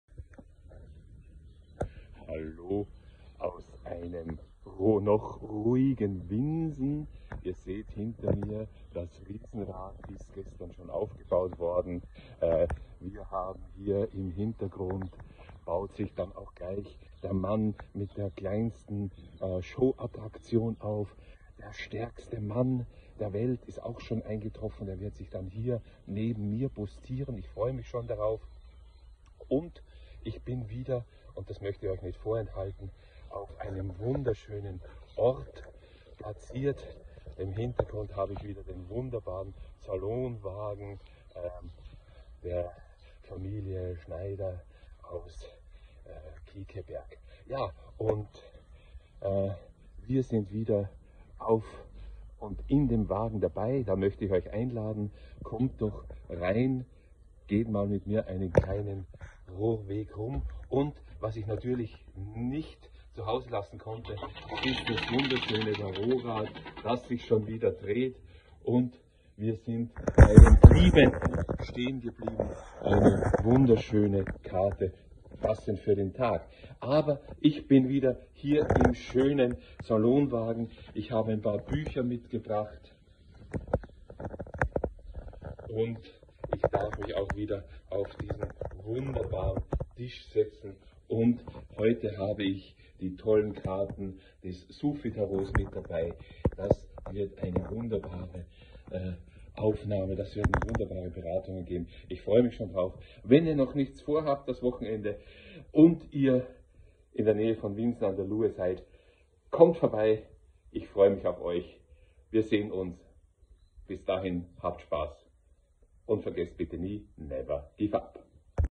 Kleiner Gruß vom Traditionellen Jahrmarkt in Winsen
Heute und Morgen ist ROE wieder in unserem stylischen Salonwagen auf dem Hugo-Haase.Jahrmarkt unterwegs. Er freut sich über euren Besuch!